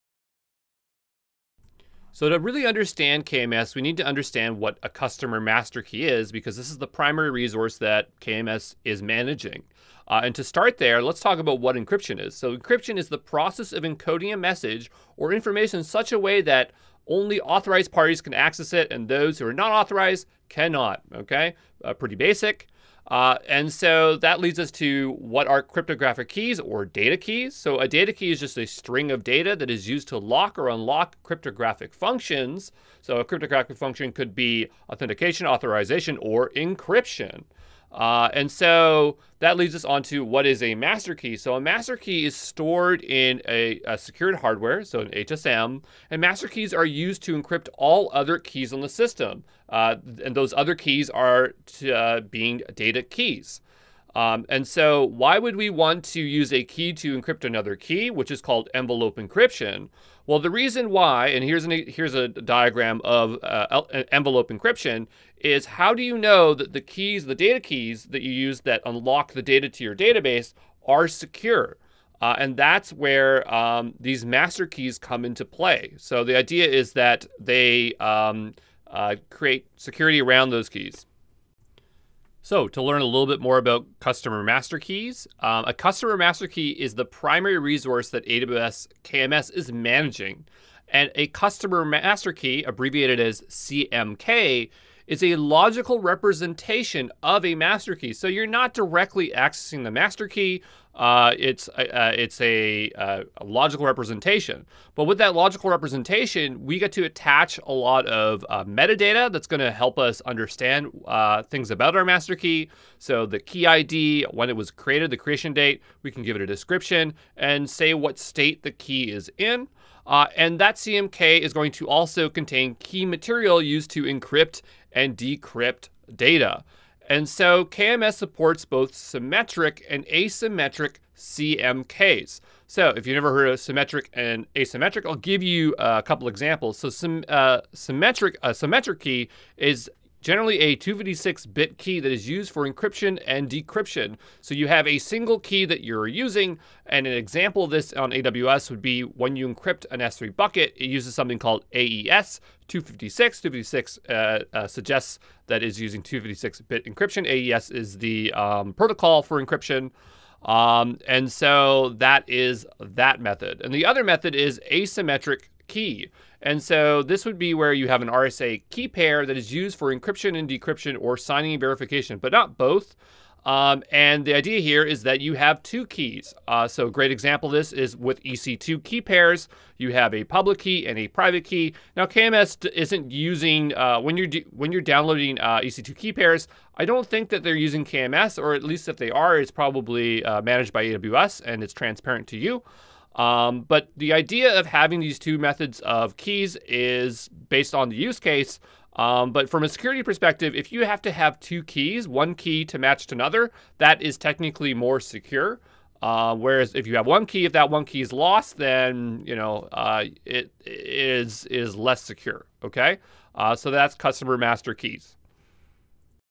Customer Master Keys (CMK) in AWS KMS - AWS Course
Understand the concept of Customer Master Keys (CMK) in AWS KMS and how they provide encryption flexibility.